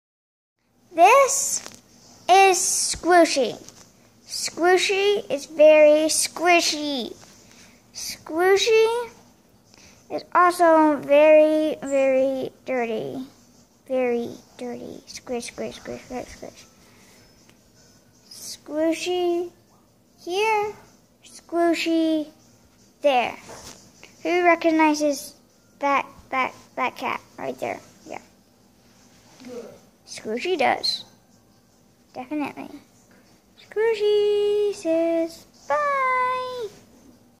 Meet Squooshi
Meet Squooshi the squishy. He is very sticky and dirty.